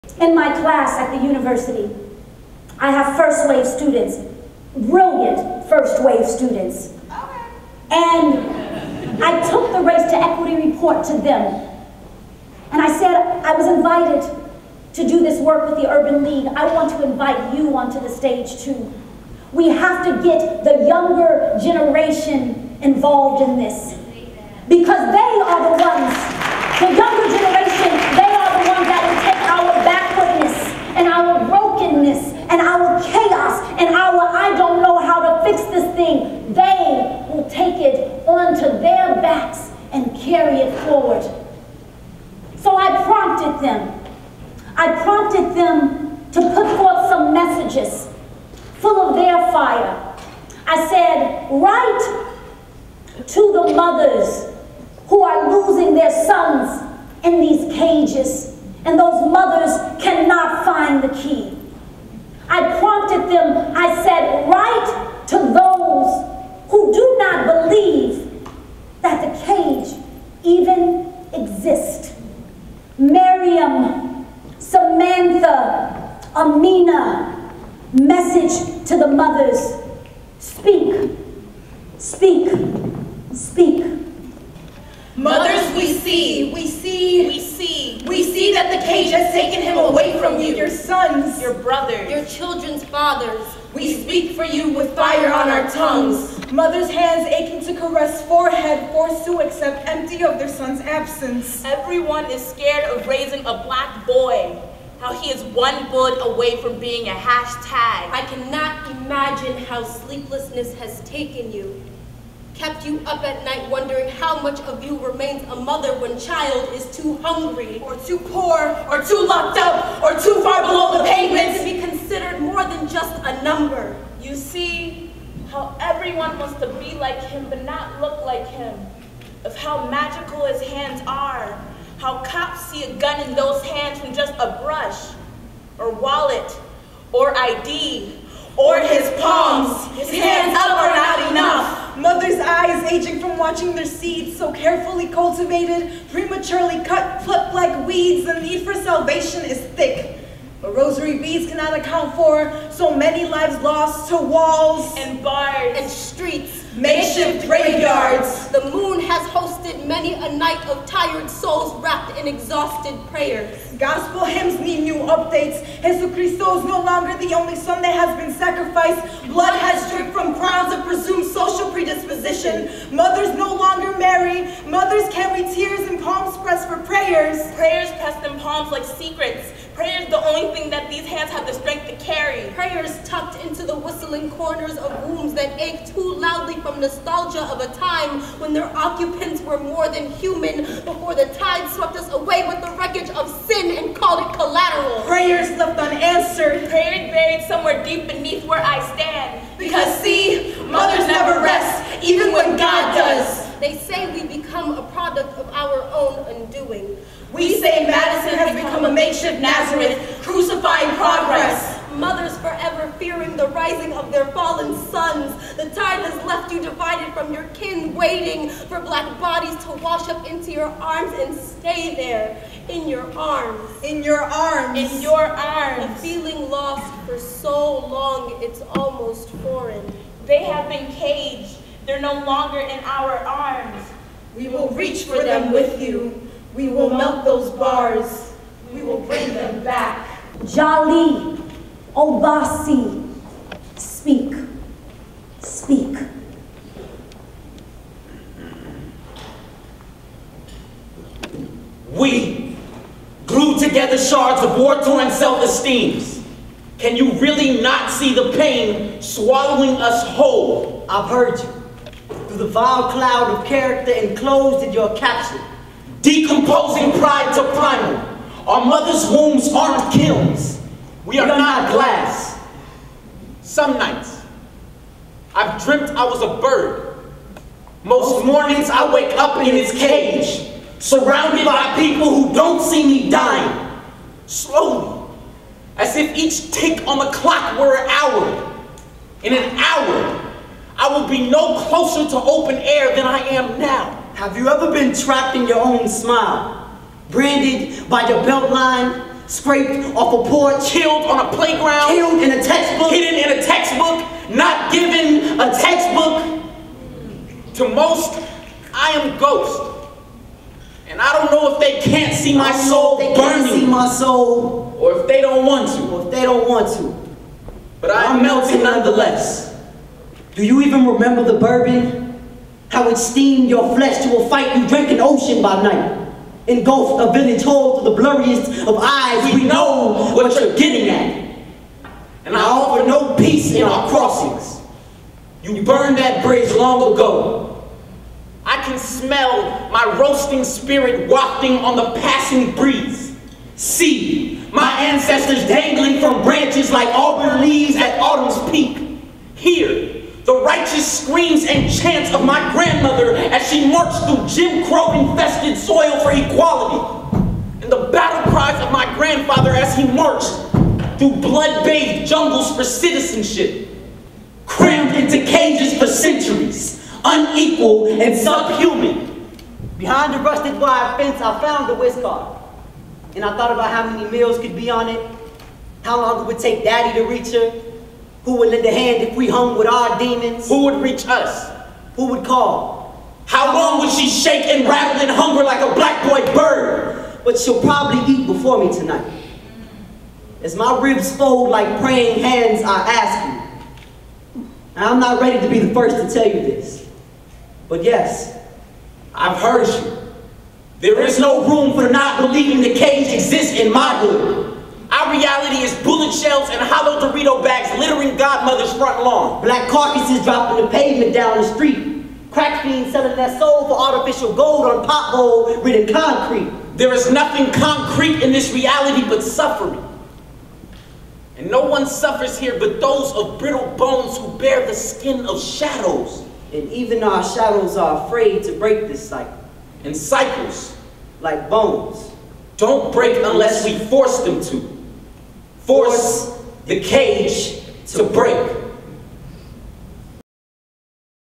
Rain Wilson A Piece of Sky Act 2 Part 2 First Wave Poets
A Piece of Sky Act 2 Part 2 features UW-Madison First Wave Poets speaking to Mothers with Sons, and to those who do not believe that the cage even exists. 7:21 minutes in length.